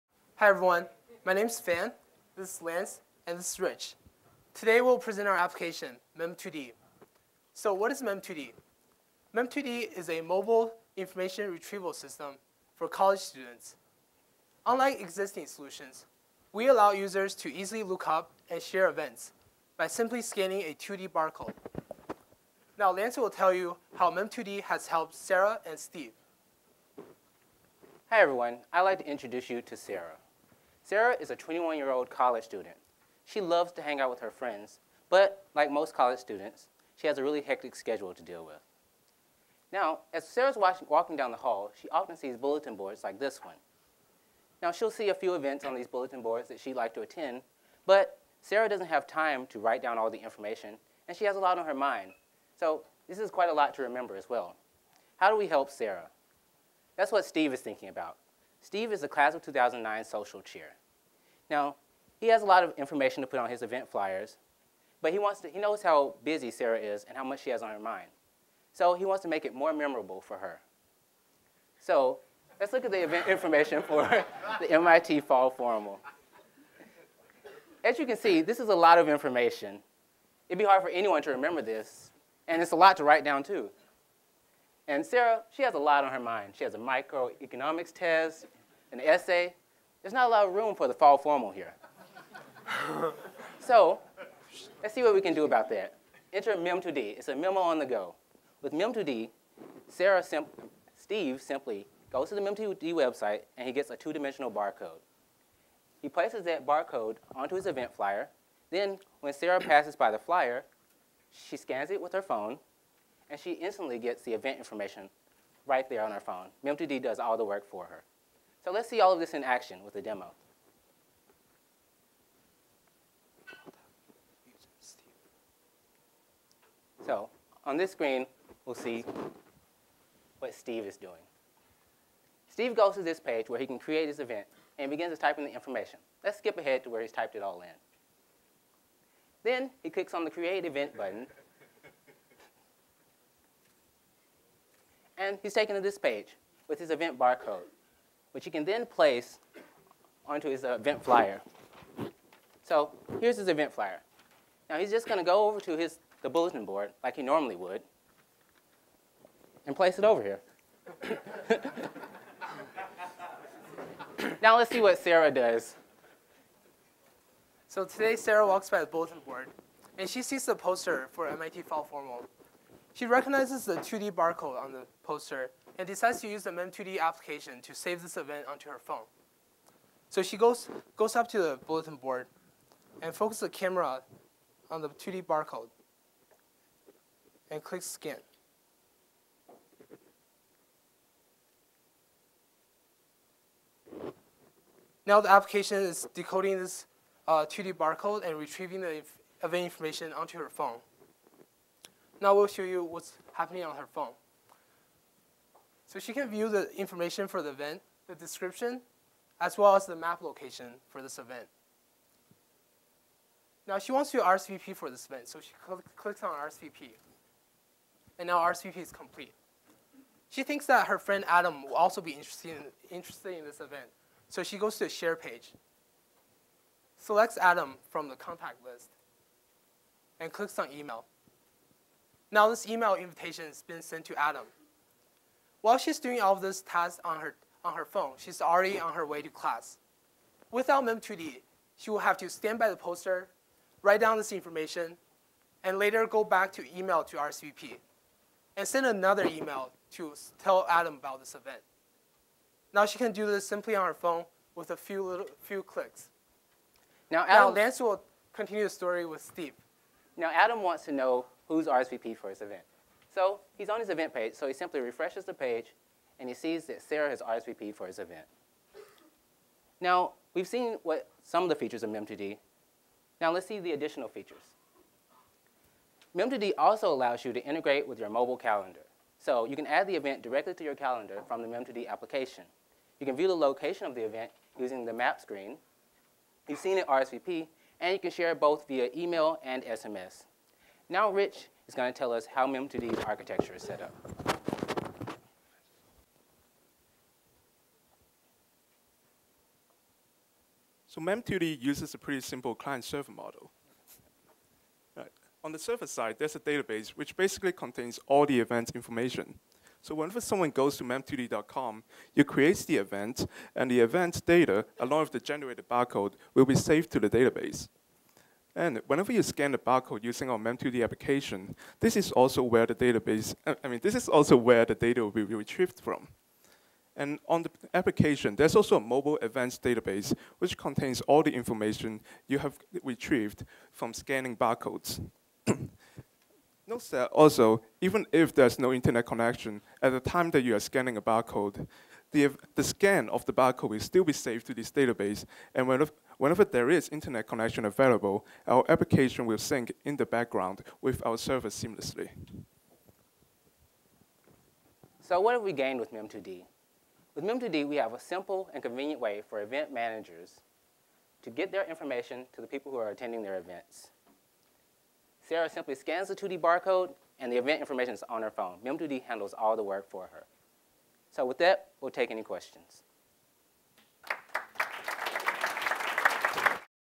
Mem2D: MIT class presentation
Memento presentation.  Hal Abelson's MIT mobile applications class, December 2008.  Presentation by Fan Yang, Rich Chan, Lance Collins
Mentors: Paul Oka, Wendy Fong, Sandy Pentland. Platform: Windows Mobile.

Mem2D is a mobile information retrieval system for college students which, unlike existing solutions, focuses on allowing users to easily look up and share events by simply scanning a 2-D barcode.